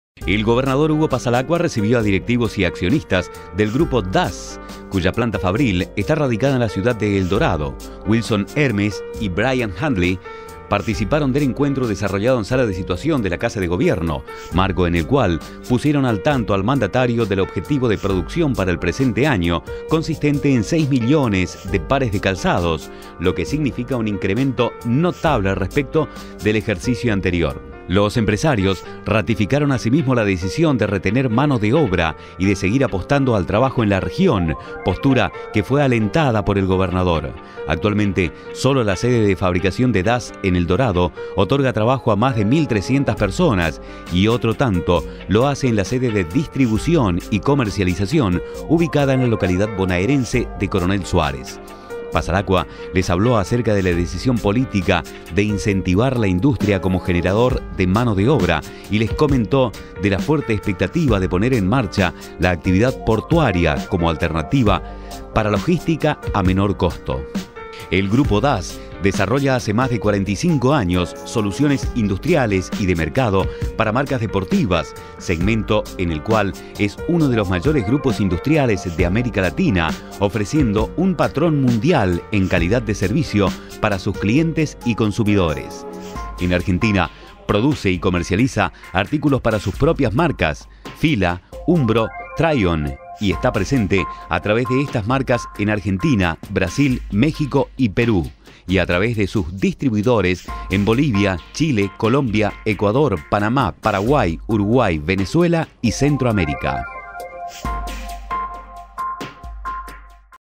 Passalacqua recibió a directivos de la empresa Dass
La firma de origen brasileño está radicada en Misiones desde 2007, y se dedica a la fabricación de calzados deportivos. La sede de  Eldorado ofrece mano de obra a más de 1300 personas.

El gobernador Hugo Passalacqua recibió a directivos y accionistas del Grupo Dass, cuya planta fabril está radicada en la ciudad de Eldorado. Vilson Hermes y Brian Handley, participaron del encuentro desarrollado en Sala de Situación de Casa de Gobierno, marco en el cual pusieron al tanto al mandatario del objetivo de producción para el presente año consistente en 6 millones de pares de calzados, lo que singifica un incremento notable respecto del ejercicio anterior. Los empresarios ratificaron asimismo la decisión de retener mano de obra, y de seguir apostando al trabajo en la región, postura que fue alentada por el gobernador. Actualmente, solo la sede de fabricación de Dass en Eldorado otorga trabajo a más de 1300 personas, y otro tanto lo hace en la sede de distribución y comercialización, ubicada en la localidad bonaerense de Coronel Suárez.

Passalacqua les habló acerca de la decisión política de incentivar la industria como generador de mano de obra, y les comentó de la fuerte expectativa de poner en marcha la actividad portuaria, como alternativa para logística a menor costo.

El Grupo Dass desarrolla, hace más de 45 años, soluciones industriales y de mercado para marcas deportivas, segmento en el cual es uno de los mayores grupos industriales de América Latina, ofreciendo un patrón mundial en calidad de servicio para sus clientes y consumidores.

En Argentina produce y comercializa artículos para sus marcas propias Fila, Umbro, Tryon; y está presente a través de estas marcas en Argentina, Brasil, México y Perú y a través de sus distribuidores en Bolivia, Chile, Colombia, Ecuador, Panamá, Paraguay, Uruguay, Venezuela y Centroamérica.